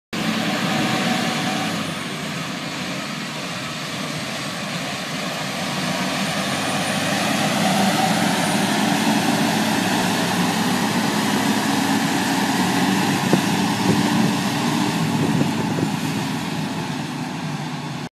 2005 DEERE 250D For Sale
Description: 8.1 L DEERE Diesel, 25 Ton Capacity, Good Brakes, Good Tires, A/C & Radio, Ready for Work-Very Nice!Category: Off-Highway Trucks